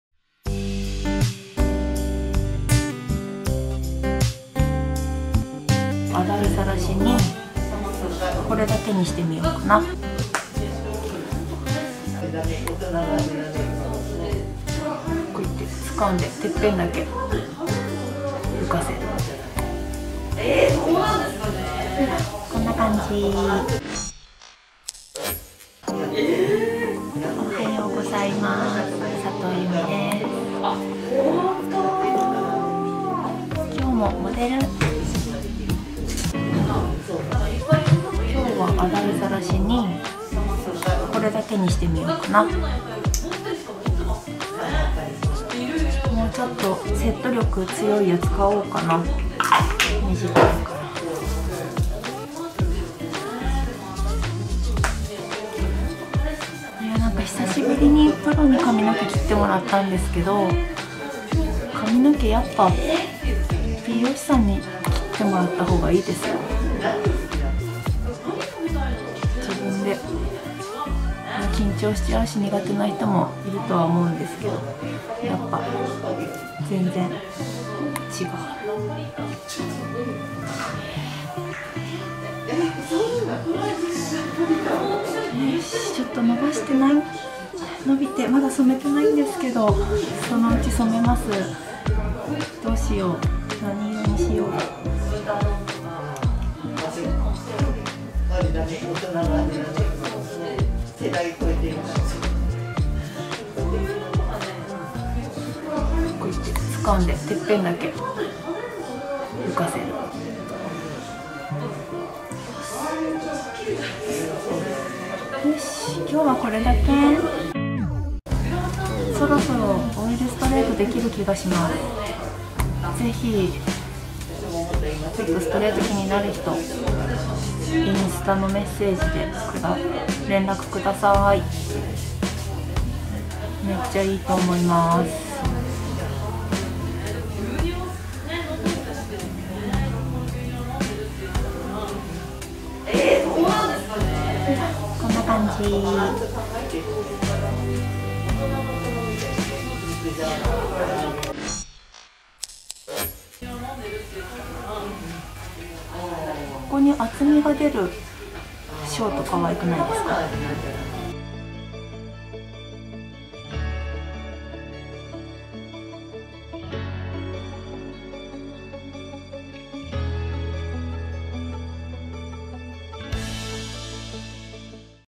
アラフォーショートヘア スタイリング剤付けるだけでアレンジ‼‼トップにはボリュームを！
札幌でフリーランス美容師をやっている佐藤有美です。

今回はスタイリング剤を付けるだけの簡単アレンジを紹介します！

このチャンネルは同年代のアラフォーの皆さんに向けて簡単なヘアアレンジなどを紹介しています！